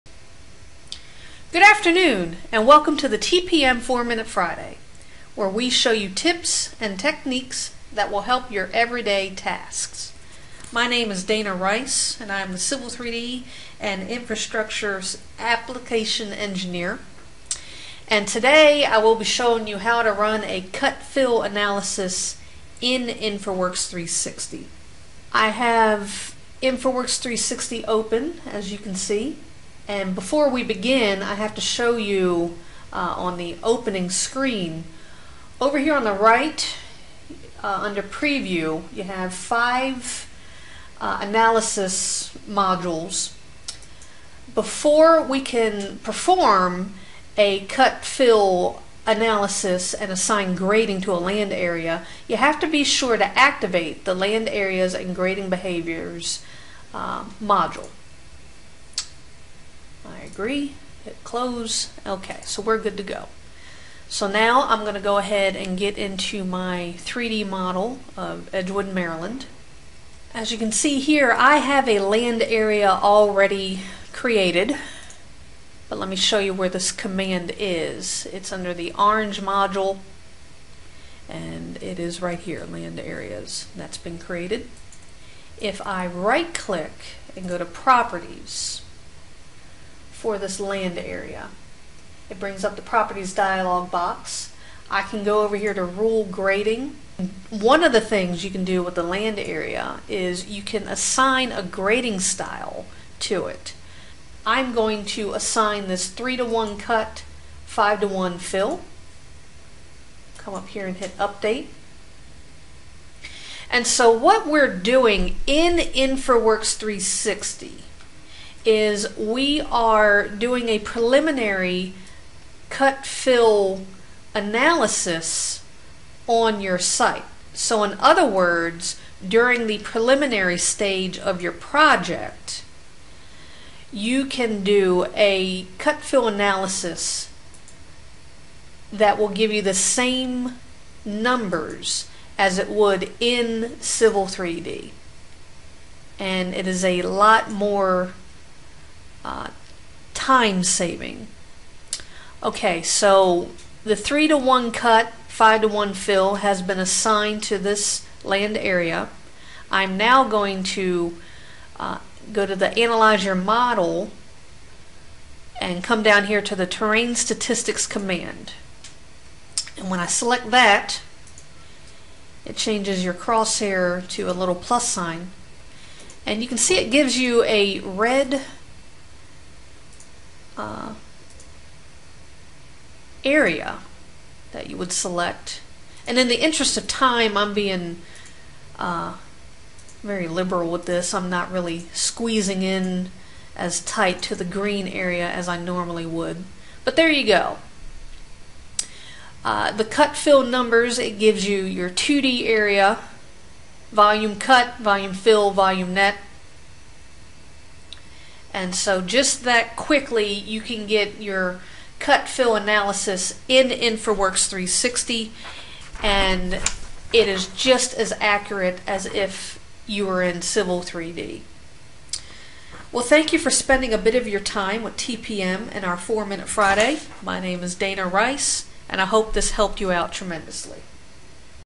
Four Minute Friday: Cut-Fill Infraworks
Infraworks has the ability to run a cut-fill analysis on your preliminary design. In this video we present this workflow.